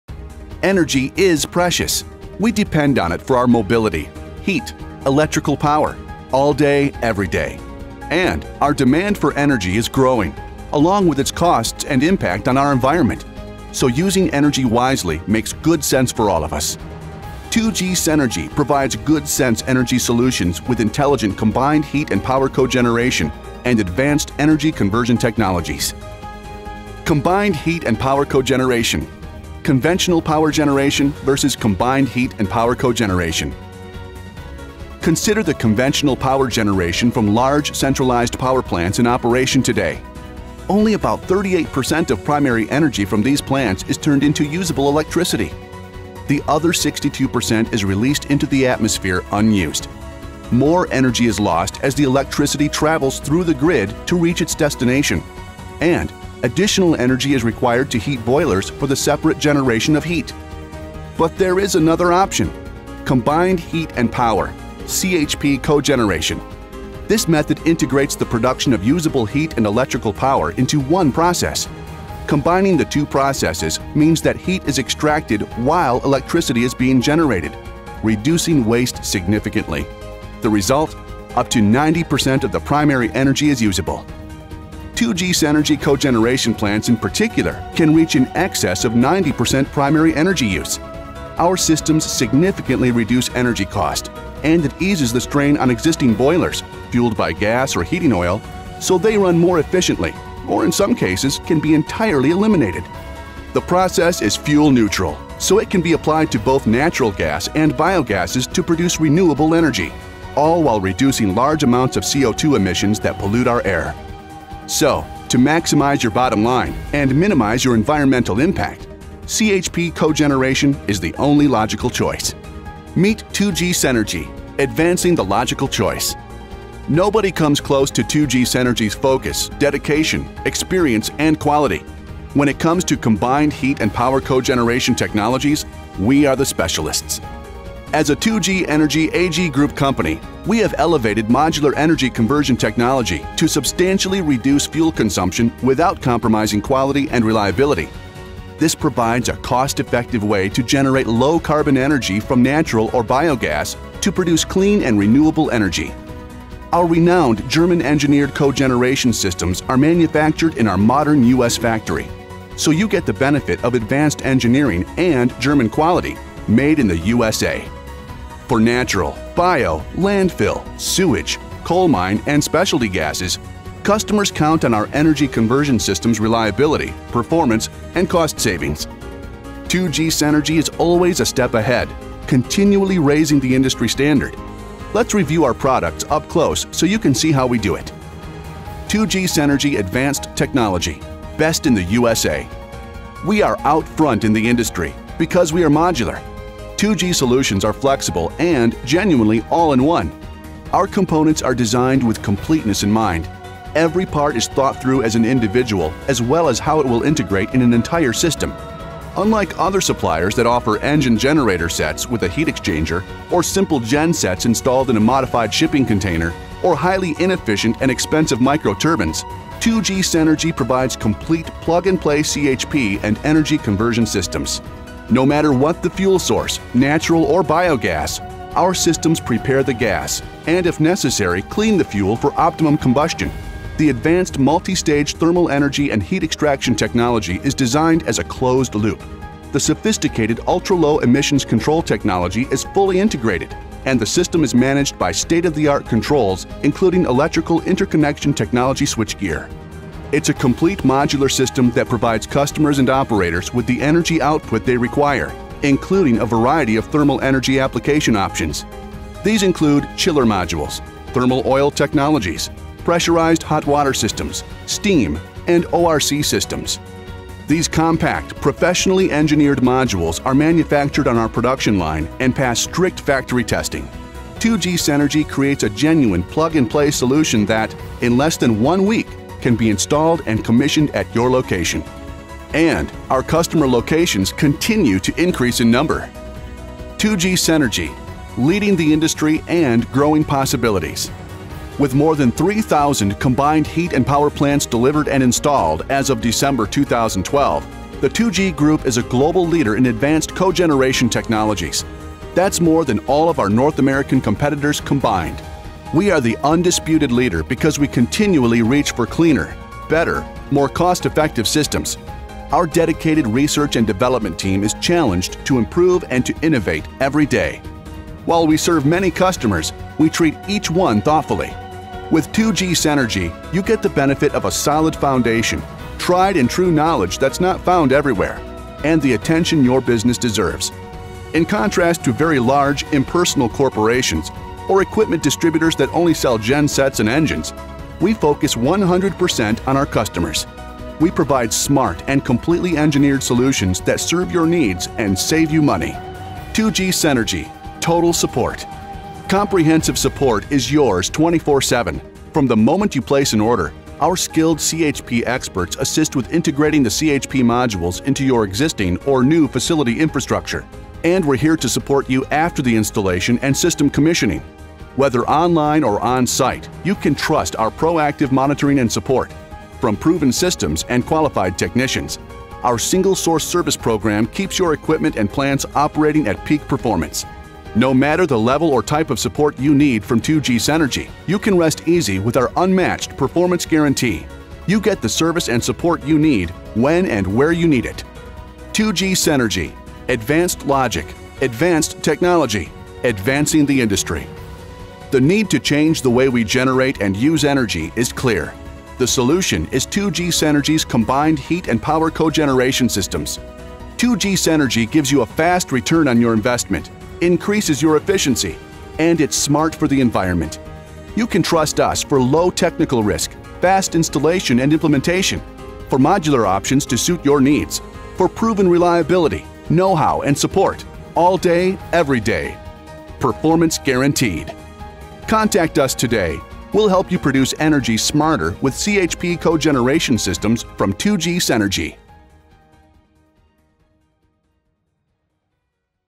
CHP Cogeneration - 2G CENERGY - Intelligent CHP Solutions
2G CENERGY is a 2G Energy AG group company. A manufacturer dedicated to the production of decentralized power generation systems, providing customers with advanced and highly efficient CHP (combined heat & power) cogeneration technologies driven by natural gas, biogas and other special gases. 2G Energy AG is the leading independent manufacturer of modular decentralized CHP power generation equipment and co-generation power plants, with a modern manufacturing facility in the USA in addition to specialized CHP manufacturing plants in Germany.